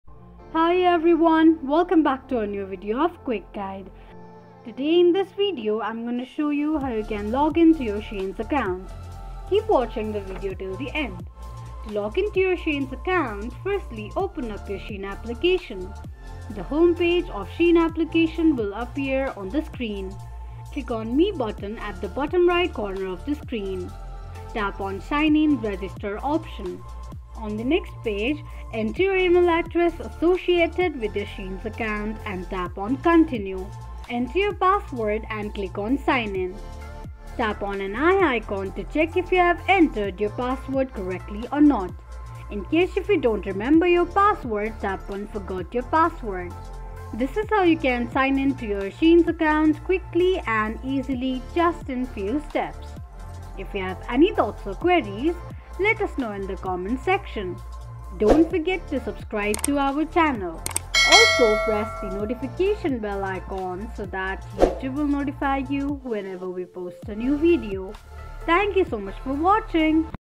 How to Login to Shein Account?
Access your fashion haven in seconds with this guide to logging in to your SHEIN account!  No need to remember complicated steps. Simply launch the SHEIN app or head to the website. Look for the profile icon or "Me" section, typically located at the bottom right corner. With a tap or click, you'll be presented with login options. Once you provide your login credentials, you'll be whisked away to your personalized SHEIN experience, ready to browse, shop, and keep track of your fashion finds!